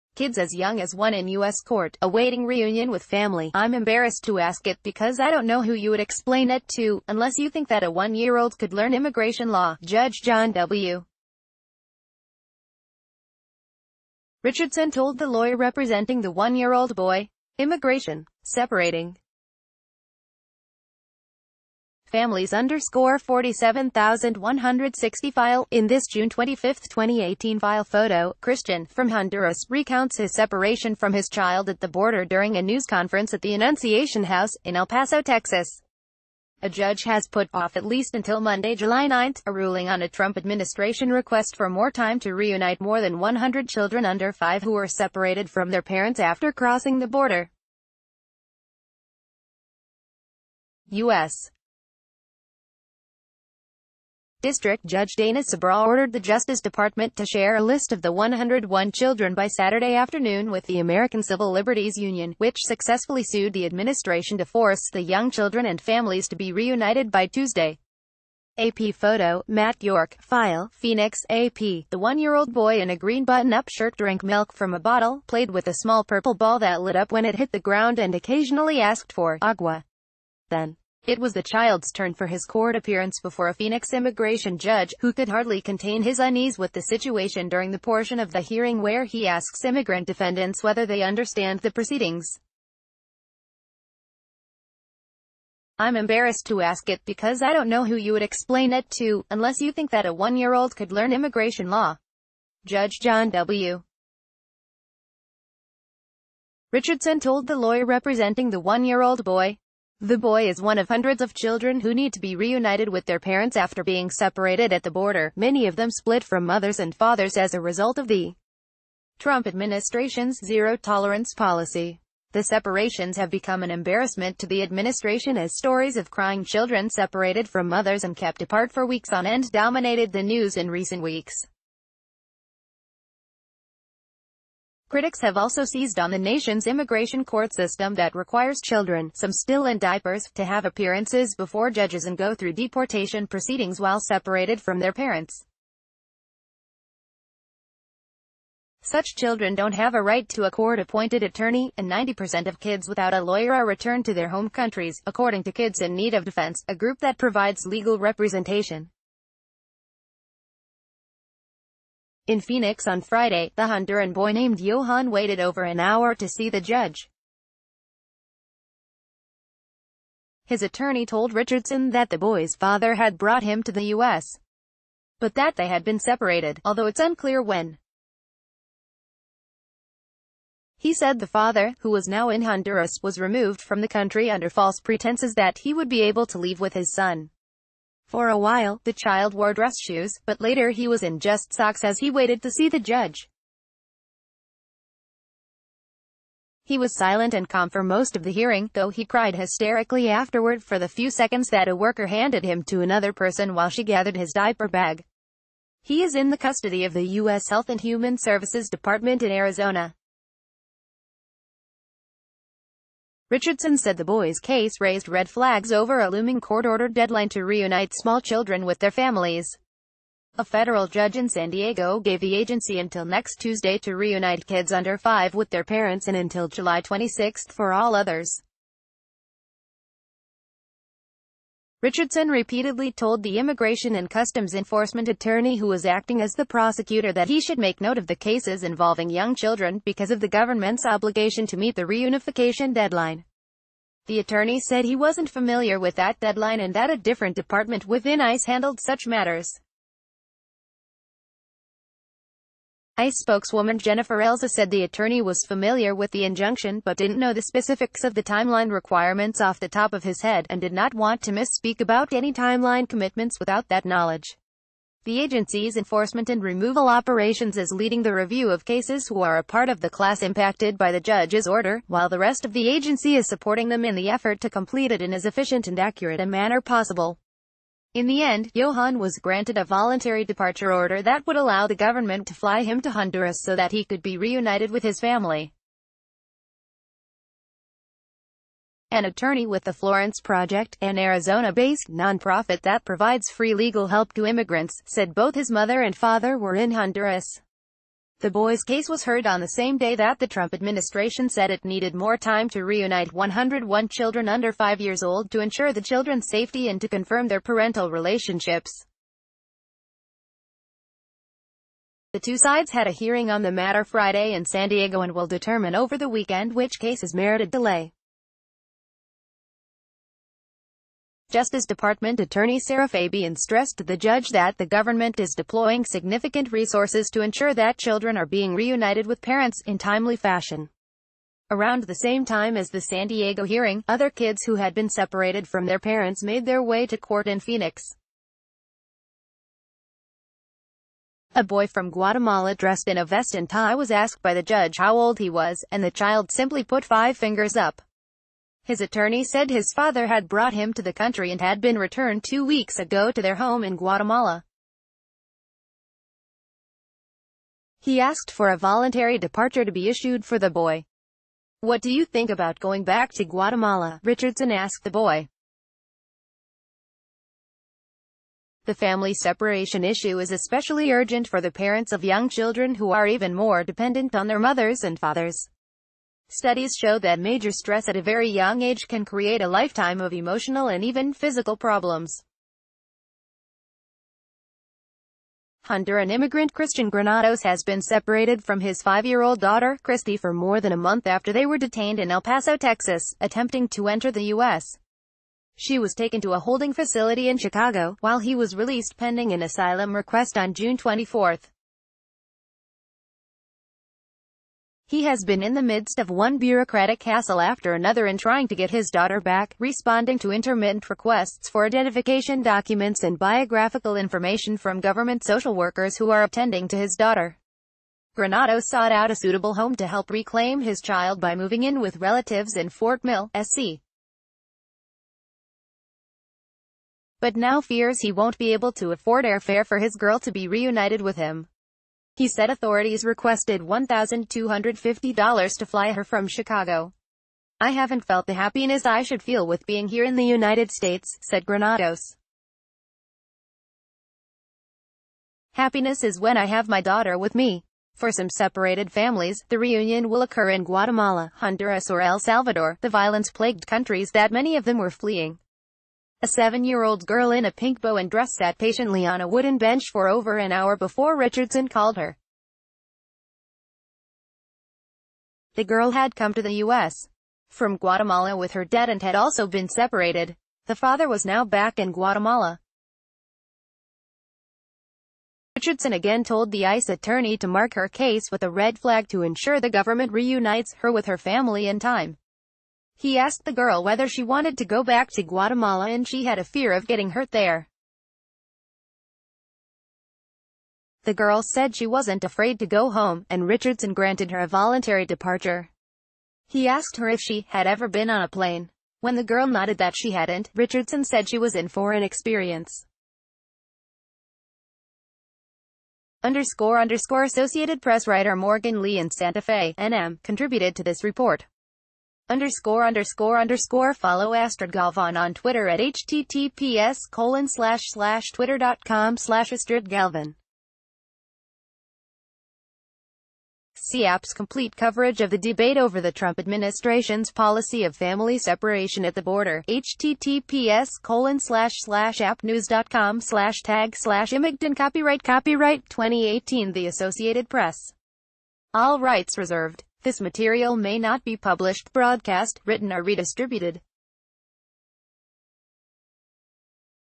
Kids as young as 1 in US court, awaiting reunion with family Tiger Time!!
Tiger Time!!